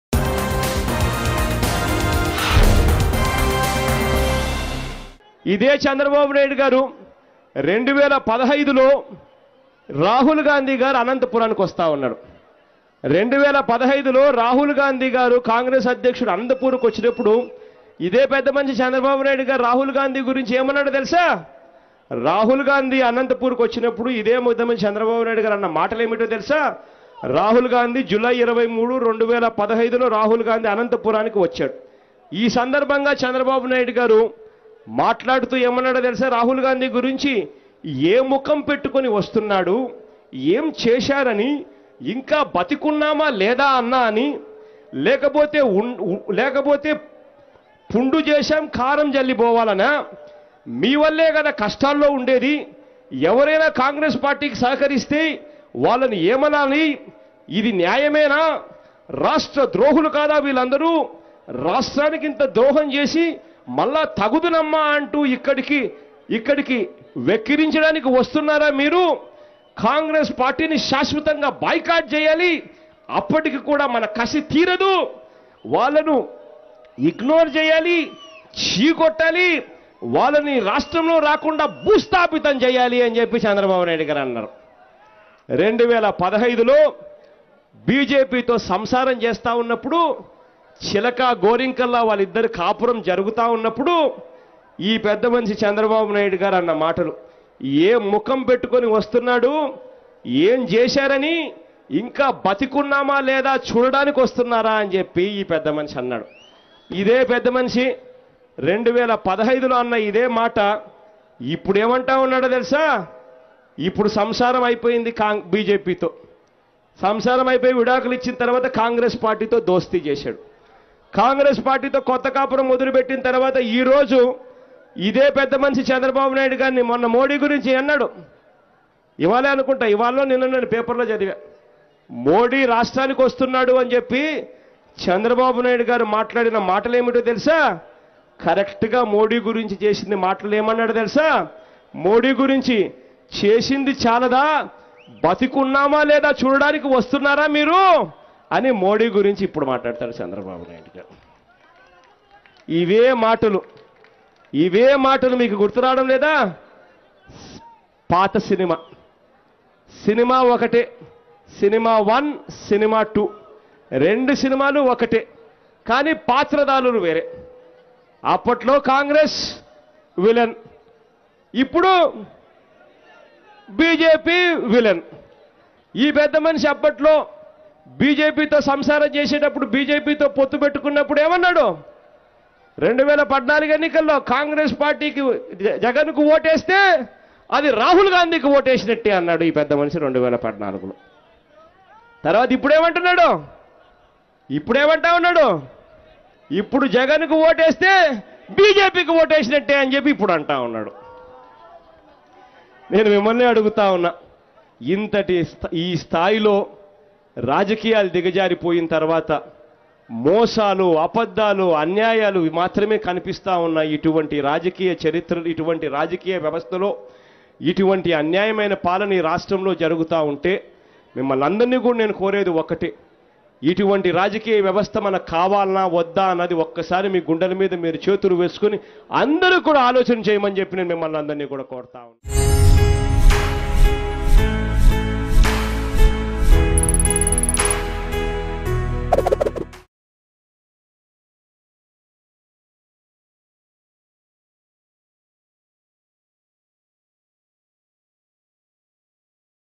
YS Jagan About Chandrababu Naidu Political Relations | Praja Sankalpa Yatra | TDP Vs YCP |Mango News
YS Jagan About Chandrababu Naidu Political Relations at Praja Sankalpa Yatra. Jagan slams Chandrababu over his alliance with Rahul Gandhi and opposing to BJP.